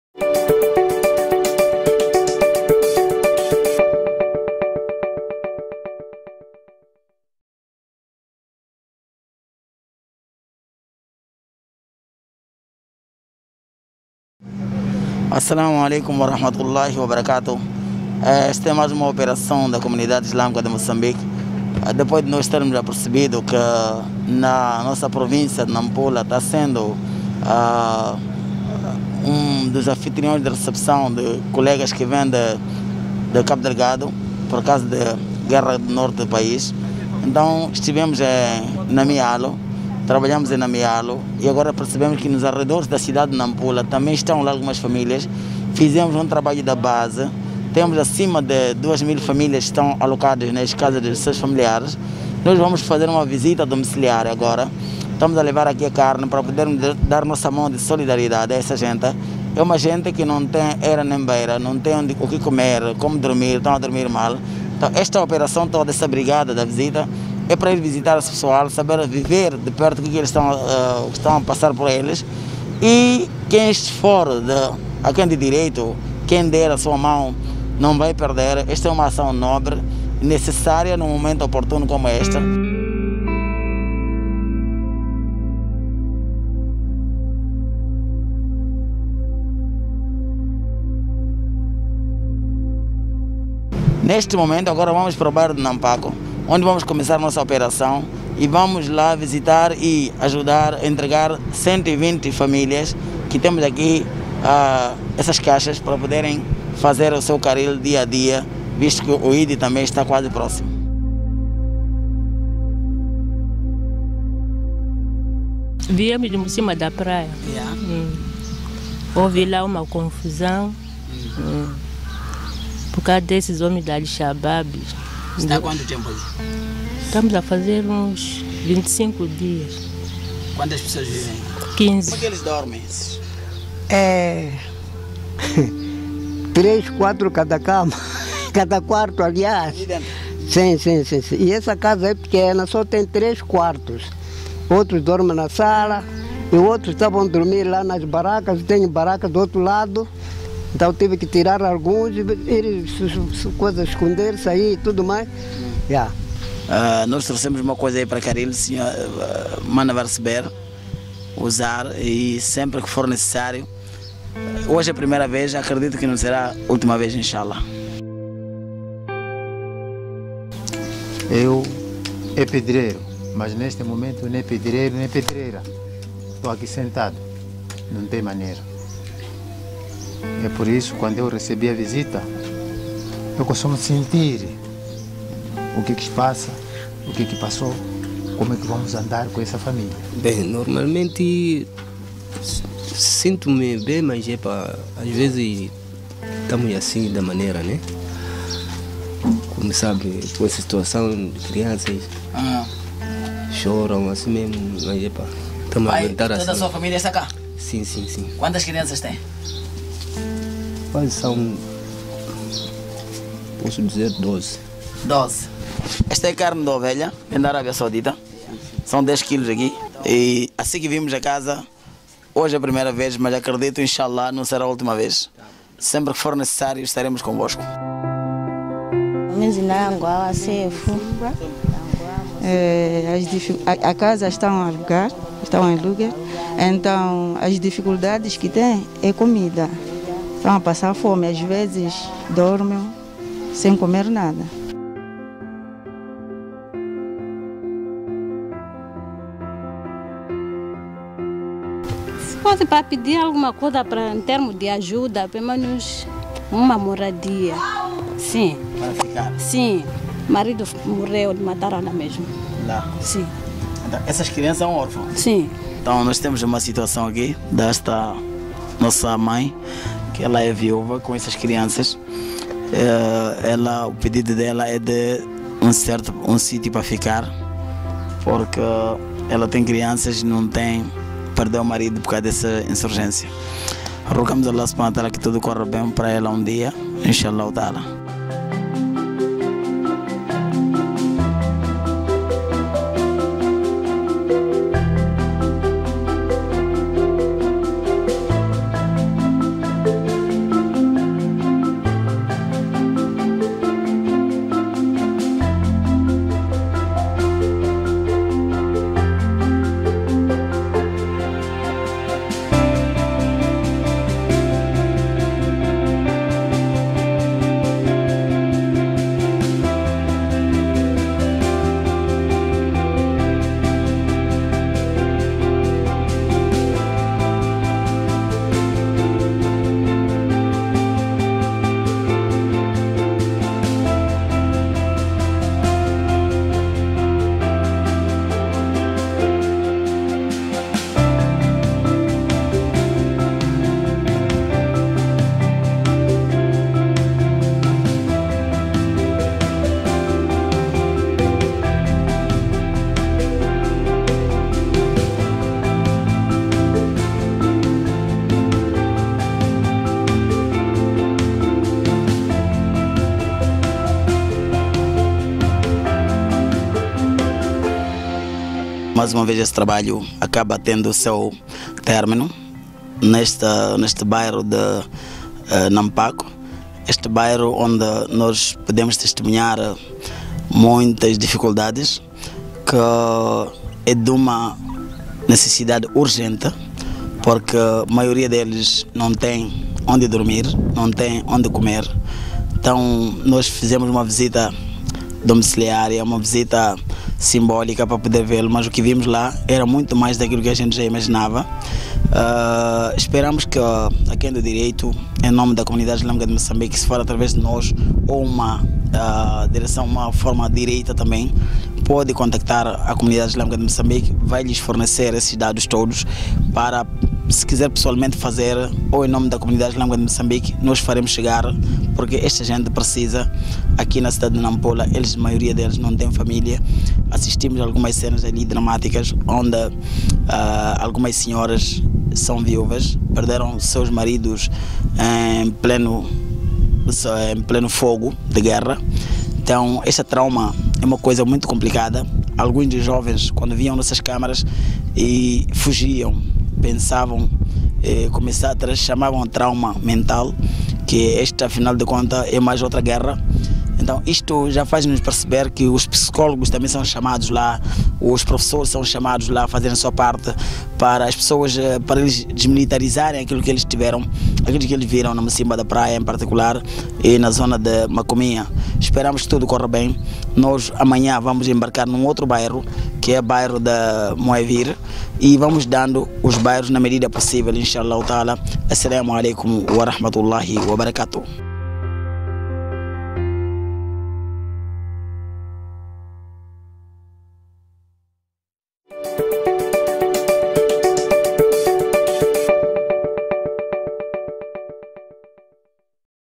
ANSTV - Distribuição de carne aos Refugiados - Nampula
REFUGIADOS

COVID-19 – Solidariedade com o próximo

Refugiados de Cabo Delgado em Namutequeliua, Muhala e Nampoco

A CIMO não poderia se manter indiferente ao momento em que os moçambicanos, particularmente os que vivem do que ganham ao dia, se encontram a atravessar. Embora necessitados e a enfrentarem dificuldades de natureza diversa este extracto da população mal se pode fazer ao mercado para ganhar o seu dia, pois o COVID-19 e a consequente decretação do Estado de Emergência não permite liberdades suficientes.

Alhamdulillah, nos  dias25 a  27 do mês em Julho a CIMO - Comunidade Islâmica de Moçambique conseguiu enviar carne ovino, roupas e sapatos onde procedeu a distribuição  para cerca de 300 famílias carentes que ali se encontram refugiadas.

Por forma a se garantir ordem e segurança procedeu-se ao levantamento de potenciais beneficiários, seguindo-se então a entrega domiciliar.

Roguemos a Allah (ST) que nos faculte mais oportunidades de puder servir as populações carentes. Aamin.